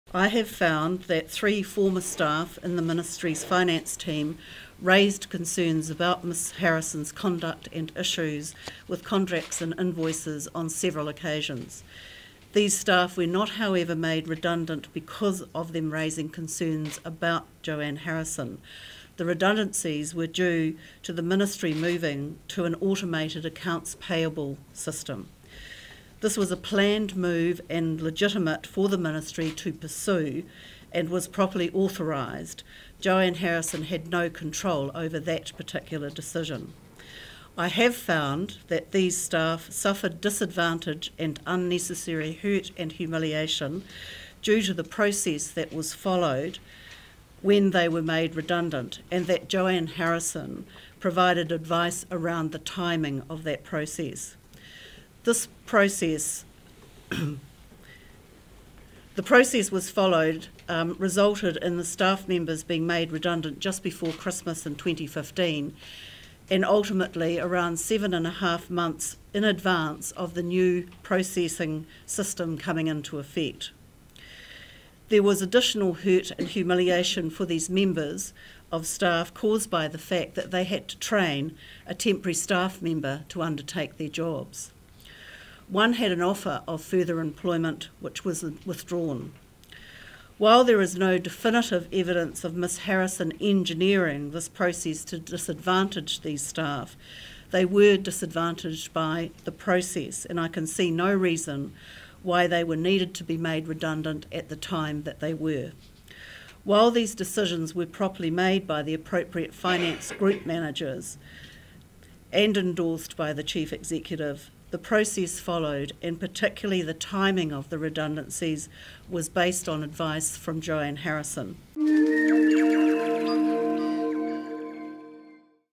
Ministry of Transport whistleblowers forced out
Ministry of Transport whistleblowers were forced out of their jobs too early and suffered "unnecessary hurt and humiliation" in a restructure process involving convicted fraudster Joanne Harrison, an investigation has found.